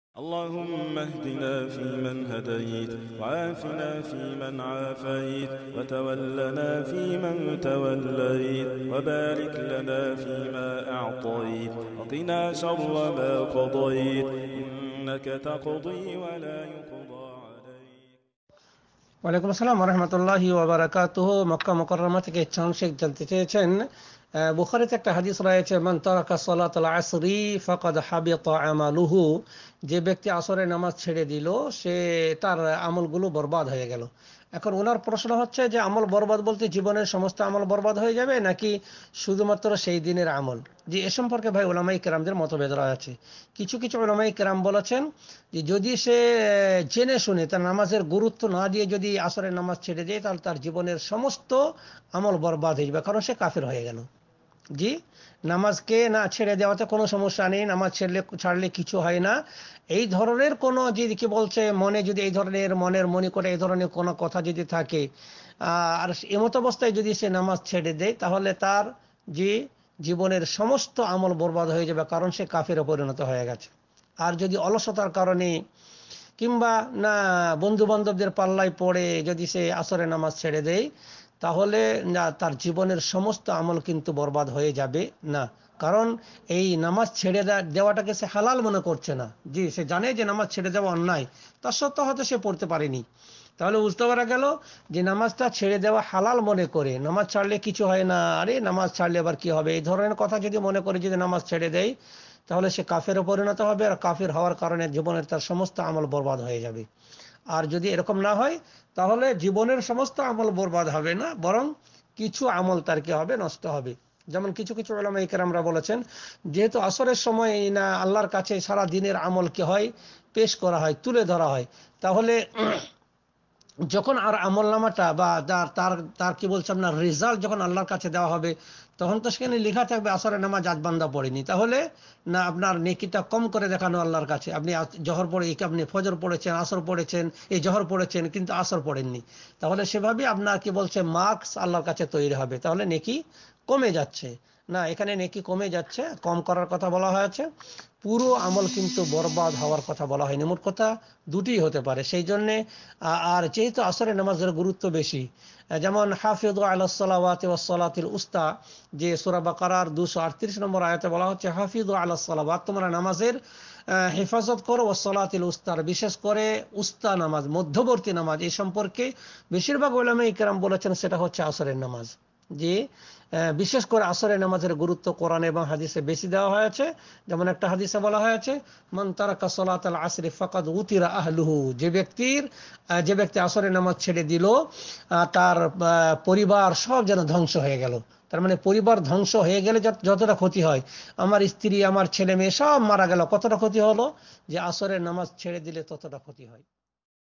আসরের সালাত ত্যাগকারী ব্যাক্তির কোন কোন আমল নষ্ট হয়ে যাবে?শায়েখ শফিউর রহমান।
আসরের সালাত ত্যাগকারী ব্যাক্তির কোন কোন আমল নষ্ট হয়ে যাবে?শায়েখ শফিউর রহমান। আপনাকে AL FURQAN media.